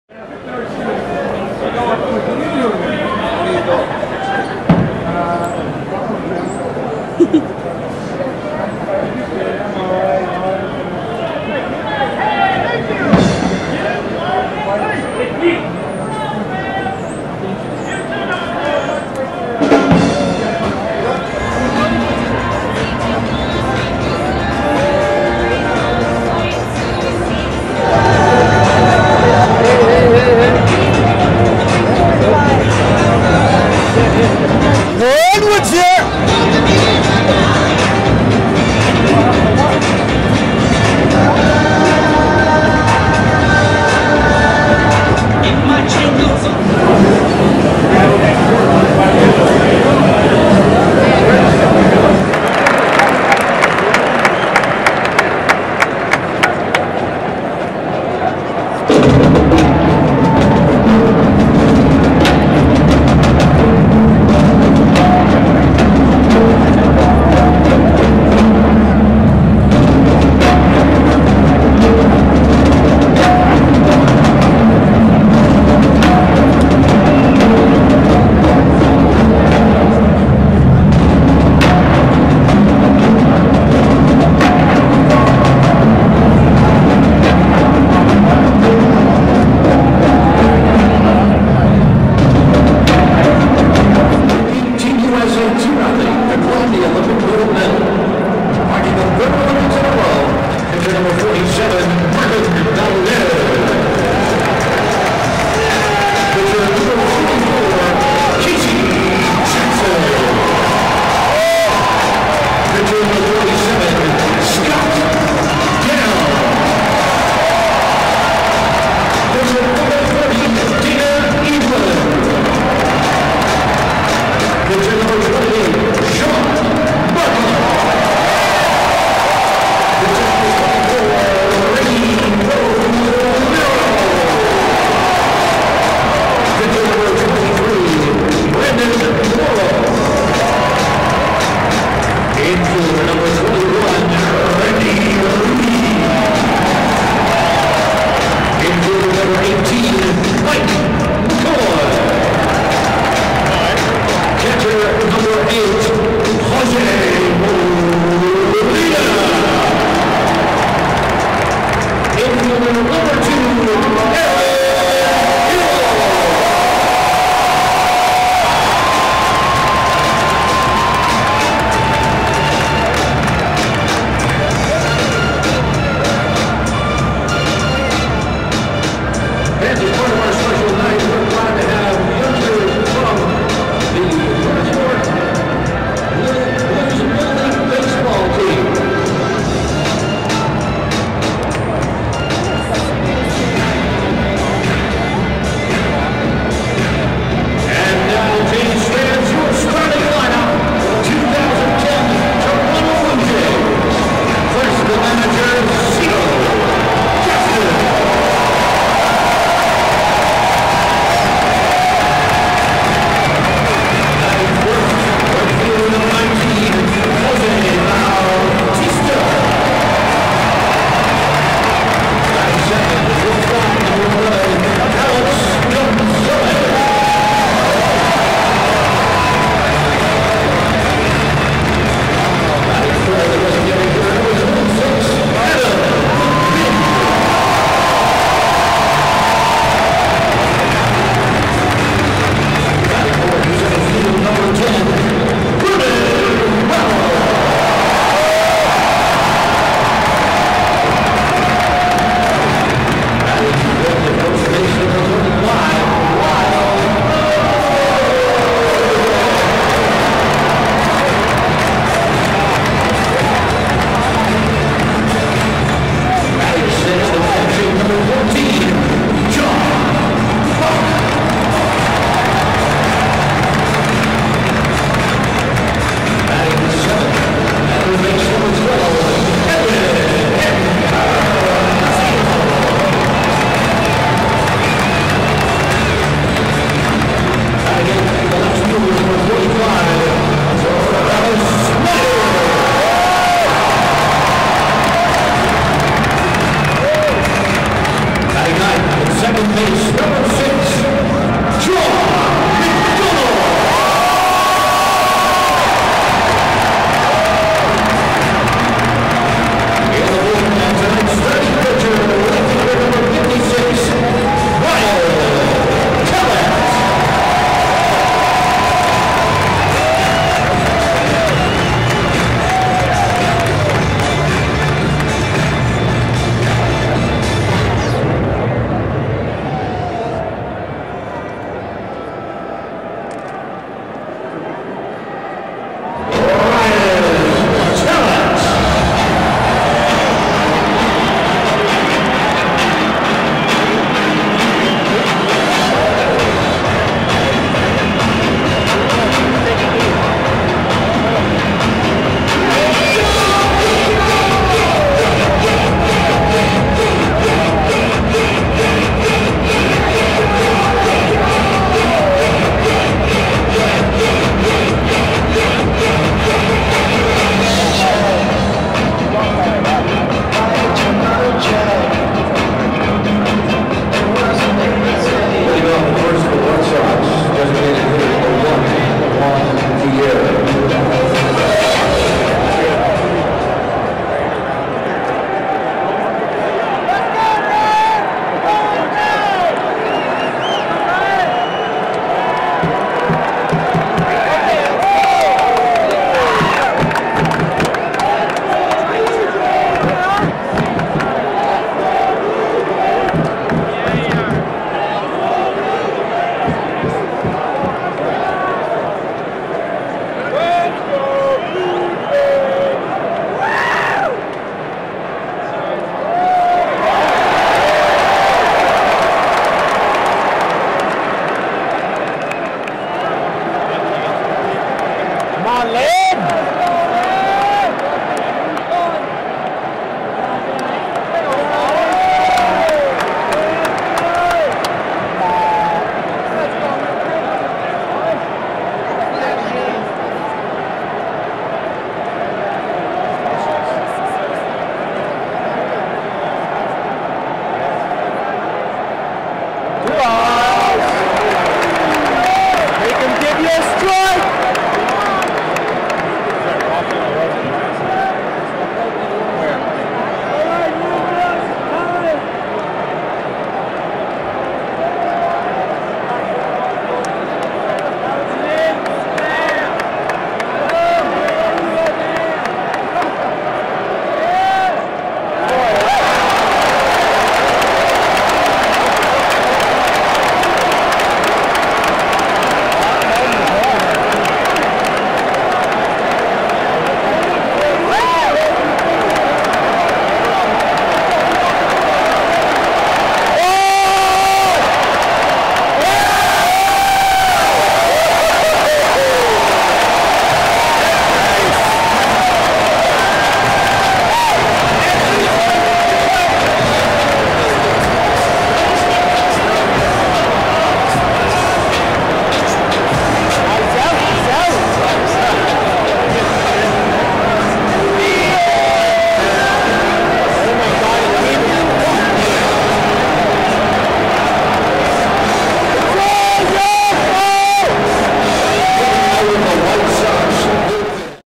Toronto Blue Jays 2010 Home Opener clips
A few clips of the Toronto Blue Jays Home Opener vs. the Chicago White Sox on April 12, 2010 at the Skydome (er, "Rogers Centre").

A lot of people couldn't even get inside the building until the 2nd inning because lineups were so long outside. Announced attendance was 46, 321.

Ends with Adam Lind up to bat after White Sox pitcher Jake Peavy was pulled in the 6th after loading the bases. Sox reliever promptly gave up a walk to Lind on four straight pitches, walking in the go-ahead run for the Jays. (They ended up losing 7-8 regardless, but this was a highlight.)

Had this on another account and transferred it over here. Mind-boggling looking back that Tallet was the starter.